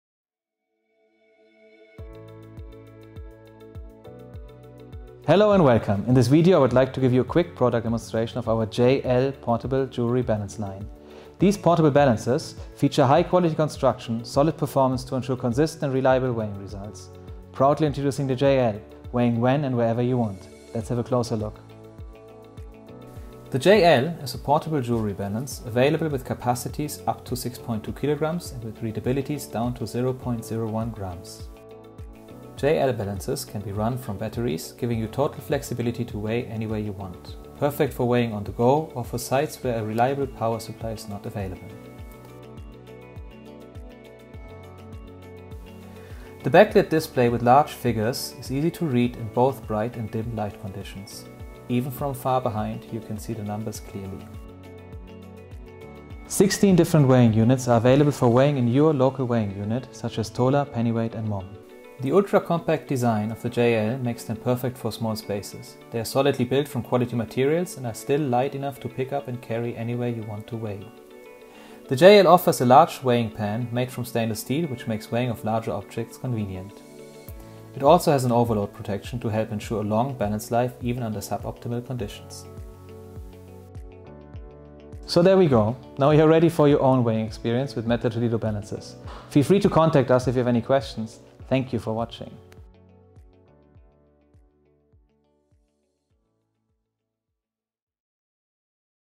JL Portable Jewelry Balances
In this video, we introduce you to METTLER TOLEDO's JL portable jewelry balance line. Enjoy a quick overview of the benefits of our easy to use, robust and affordable balances that are ideal for weighing on the go!

JL gold balances combine quality with compactness and affordability to fulfill all your simple gold weighing needs. With an ultra-compact design, JL balances are perfect for small spaces. They are solidly built, yet light enough to carry conveniently to wherever you want to weigh.

16 different weight units are available. With one touch, you can swap between two selected units: ct, g, mg, kg, pound, ounce, troy ounce, grain, pennyweight, momme, mesghal, Hong Kong tael, Singapore tael, Taiwan tael, tola, baht.